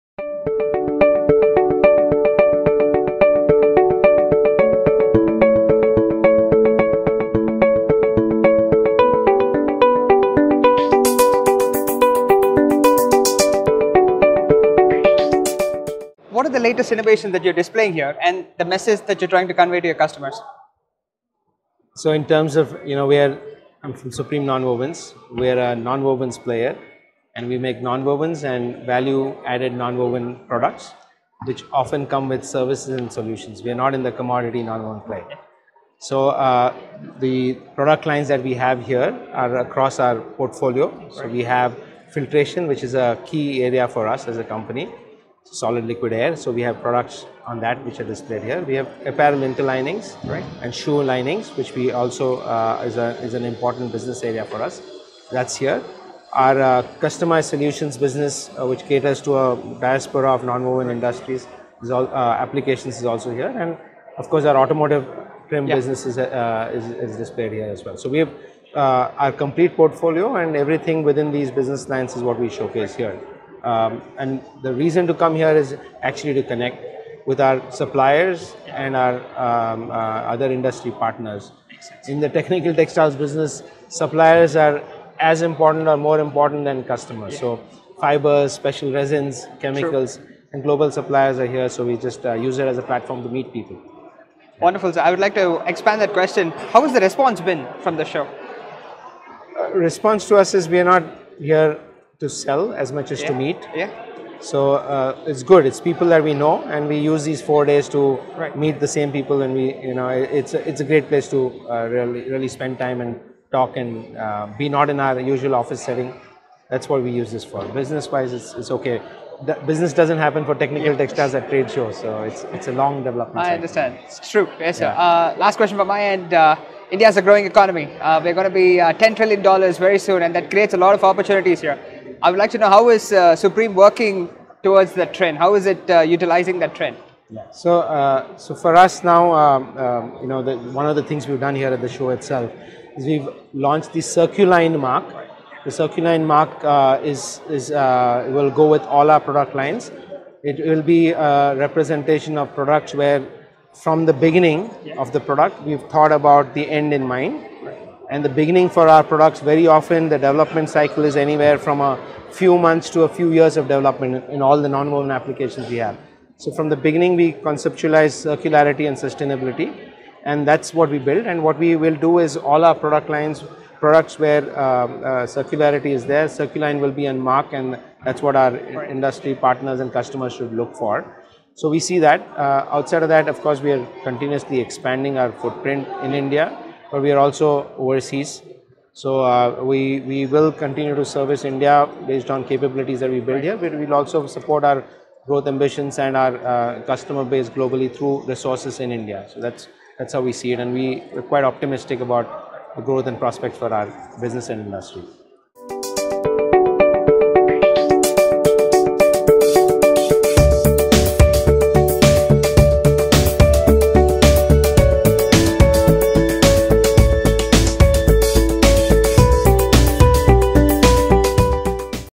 BHARAT TEX 2024 | Exclusive Interview | Amit Kavrie, Managing Director | Supreme Nonwovens
Join us for an exclusive interview with Amit Kavrie, Managing Director, Supreme Nonwovens. In this enlightening conversation we delve into the insights, innovations, and future prospects of the nonwovens industry.

From the latest advancements in technology to sustainable practices shaping the landscape, Amit Kavrie shares invaluable expertise gained from years of experience in the field. Don't miss out on this opportunity to gain a deeper understanding of the dynamic textile sector and the vision driving its growth.